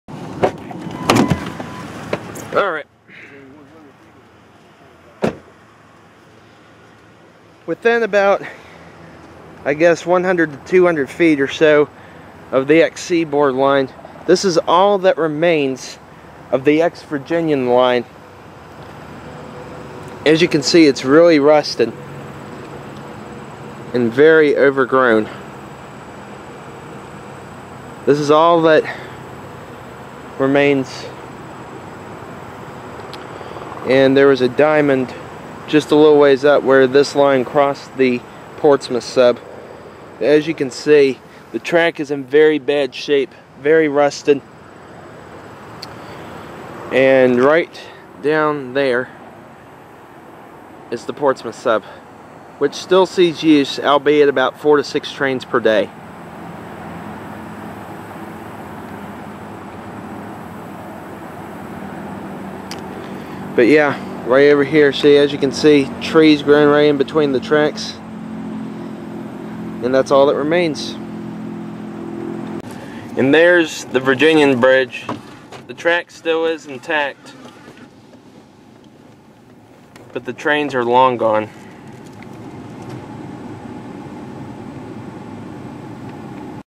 Virginian track and bridge
All that remains of the Virginian at Bowers Hill. At one time, this was where Virginian and Seaboard Air Line ran parallel to each other until just west of here, where they crossed at a diamond to run parallel again. Everything changed when the N&W bought the Virginian. As a result, I guess the N&W didn't see fit to have two parallel east-west mainlines, and this line was abandoned.

Recorded 9.2.2013